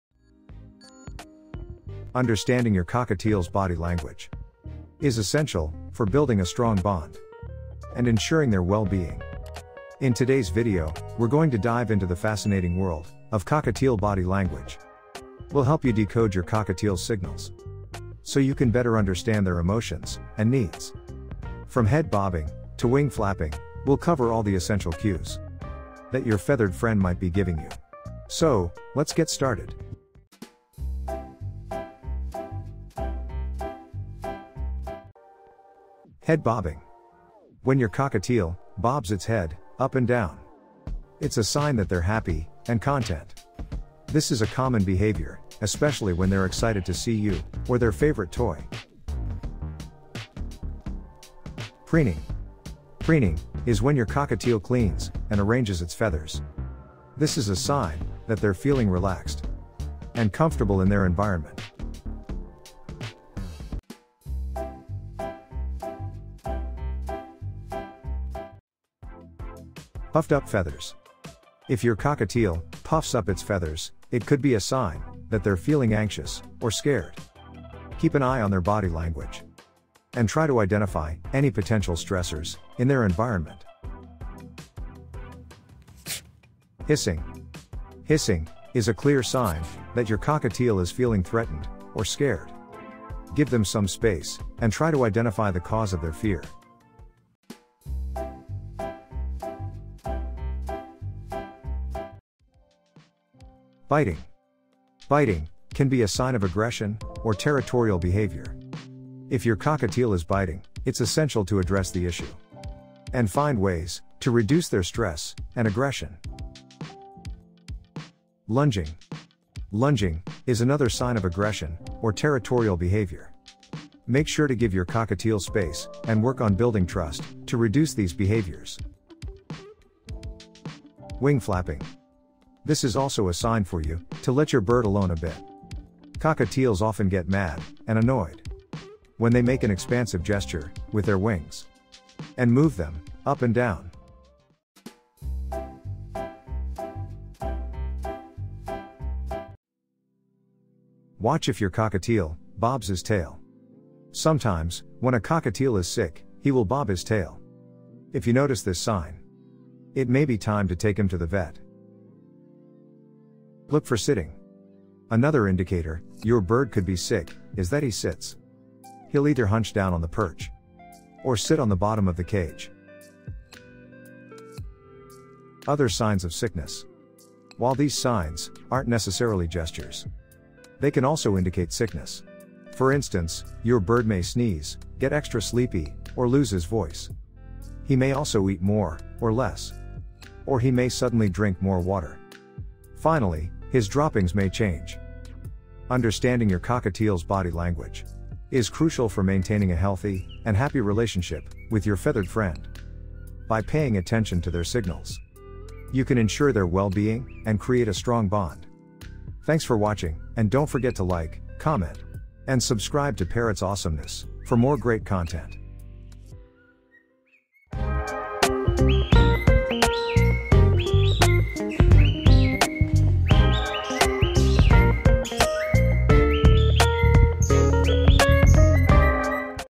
Decoding Cockatiel Body Language: Surprising Secrets Revealed!
Join us as we dive into the various signs and behaviors that your cockatiel may display, from happy and content to anxious or scared. We'll also discuss how to address any issues and build a stronger bond with your feathered friend. 🎉🦜

 -- TIMESTAMPS --
00:00 INTRO
Happy and Content
00:35 Head Bobbing
00:50 Preening

Anxious or Scared
01:08 Puffed-up feathers
01:25 Hissing

Aggressive or Territorial
01:42 Biting
01:58 Lunging
02:11 Wing flapping

Noticing Signs of Sickness
02:30 Watch if your cockatiel bobs his tail
02:43 Look for sitting
02:56 Notice other signs of sickness

03:15 Conclusion